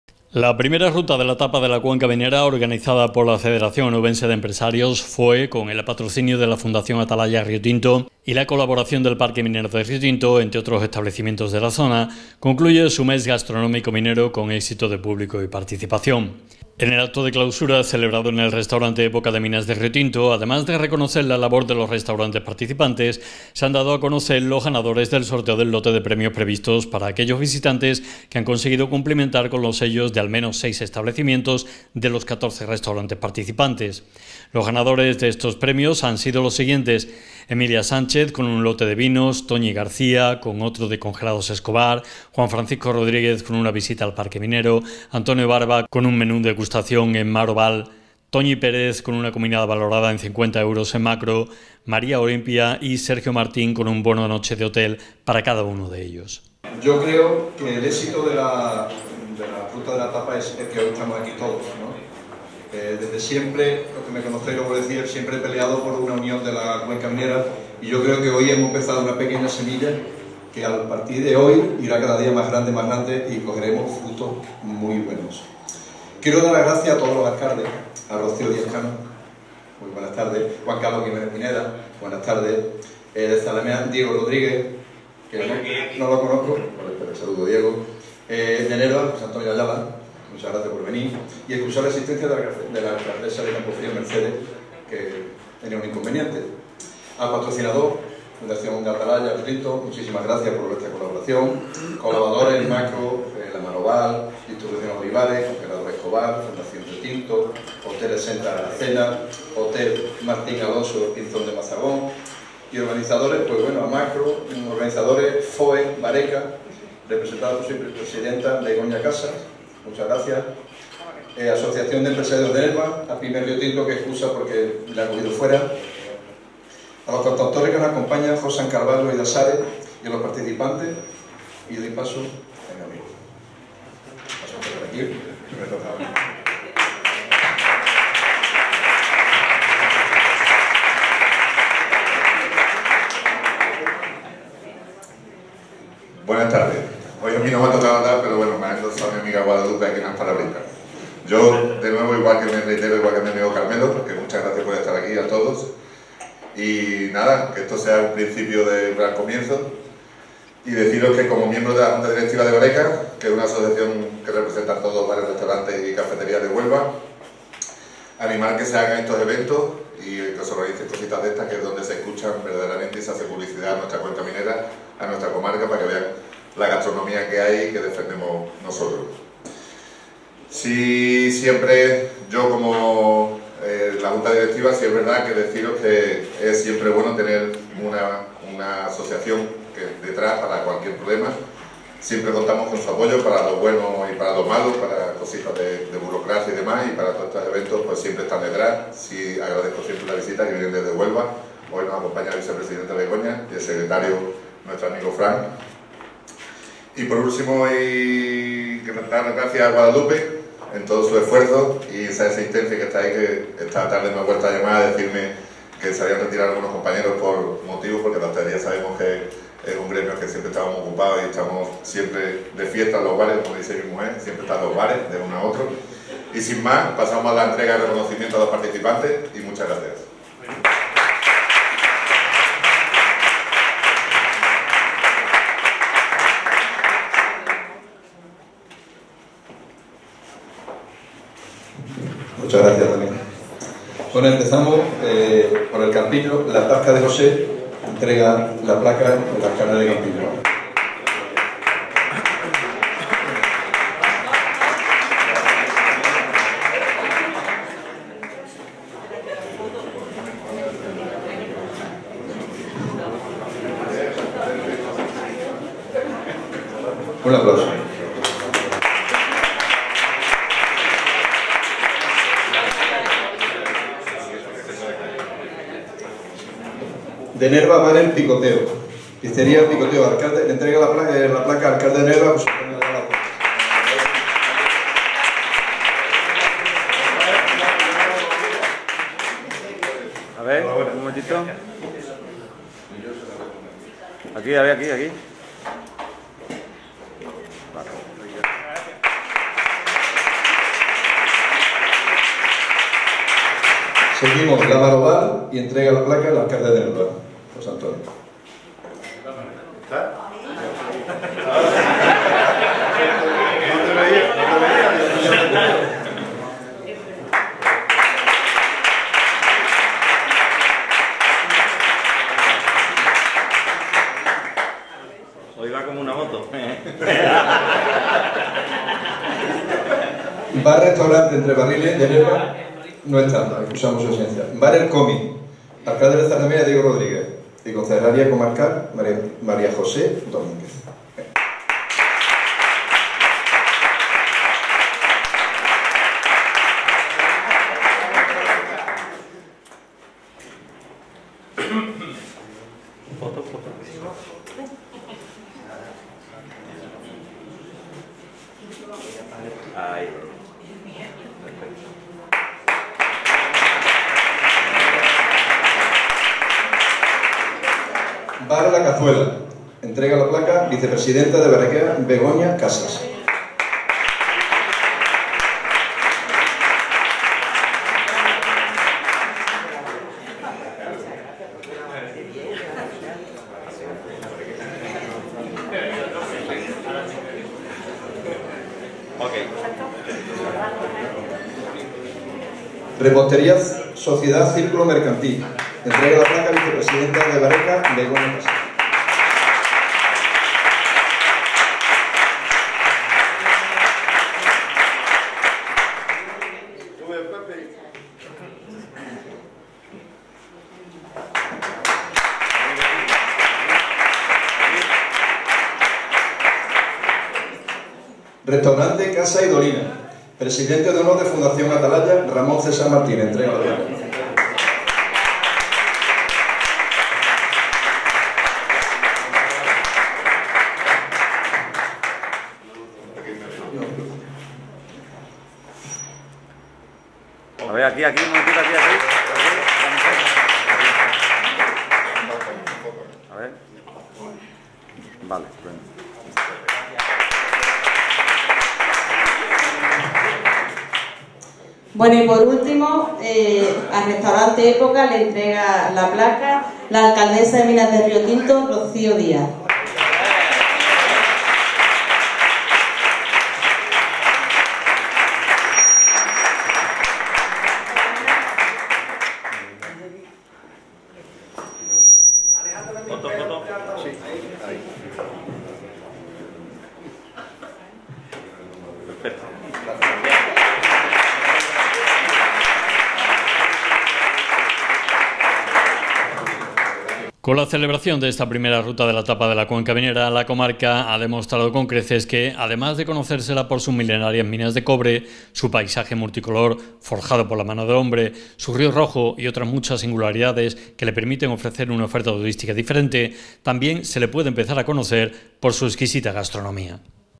Clausuran con éxito la I Ruta de la Tapa de la Cuenca Minera de Riotinto
Este vídeo trata de: Clausuran con éxito la I Ruta de la Tapa de la Cuenca Minera de Riotinto